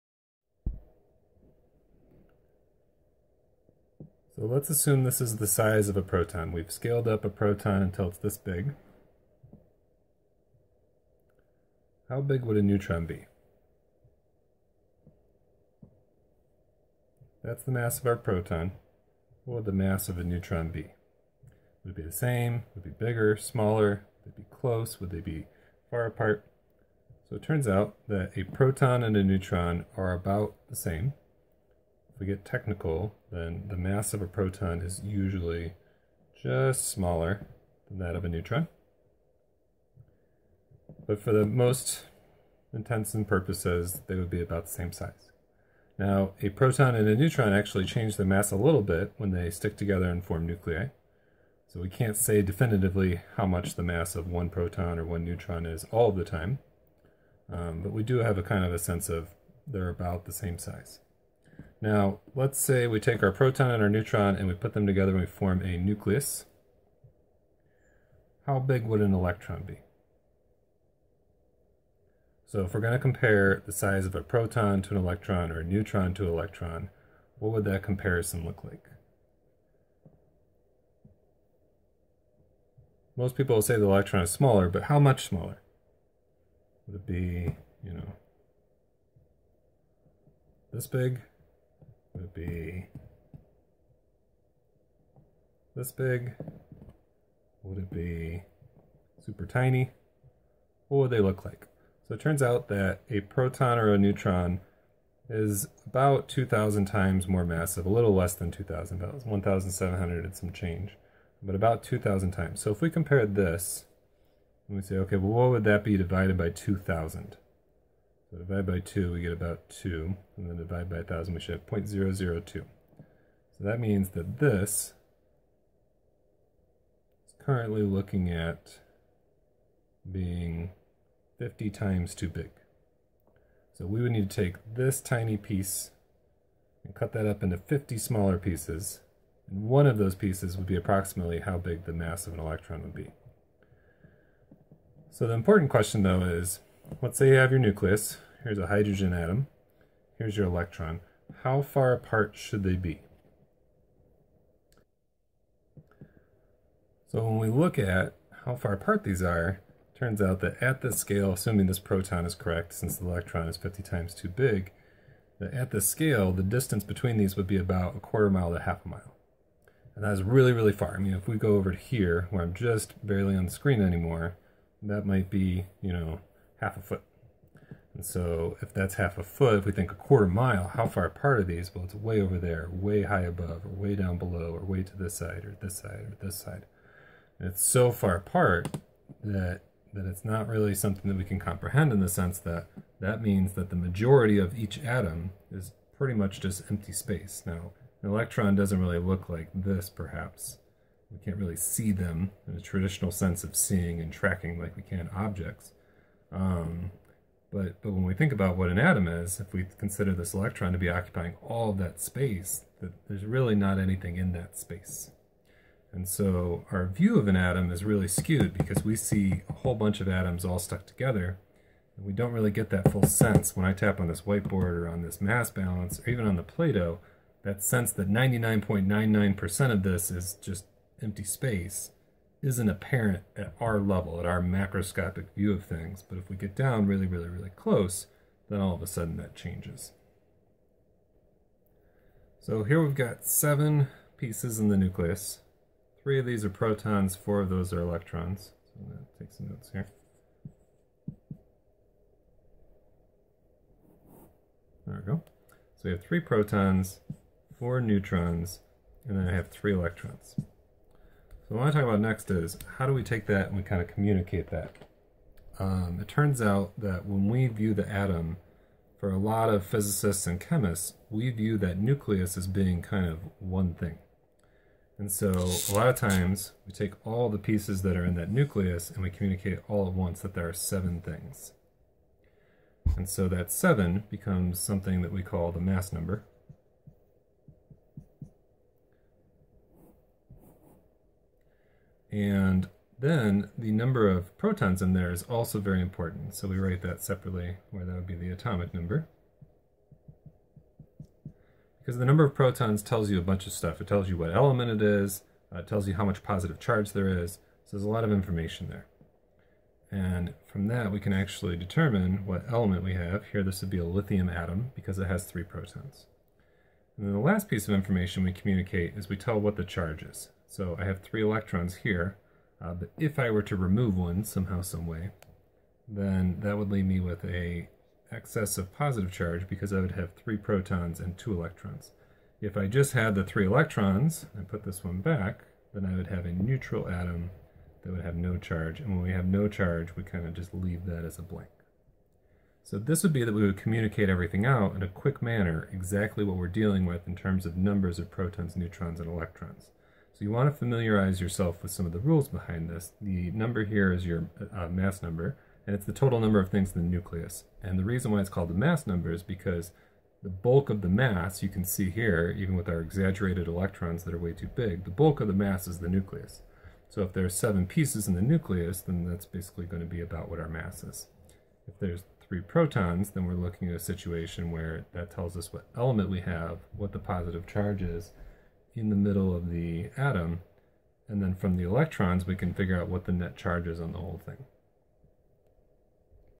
Play Doh subatomic particles
How do the relative sizes of protons, neutrons and electrons compare?
The atomic mass unit is defined as 1/12th the mass of a Carbon-12 atom. This works out that the mass of a proton or neutron is about 1 amu. The exact mass changes as the protons/neutrons bind in different nuclei. So the proton in a H atom has a different mass than the proton in a neon atom.
Proton - about 1 amu
Neutron - about 1 amu
Electron - about 0.005 amu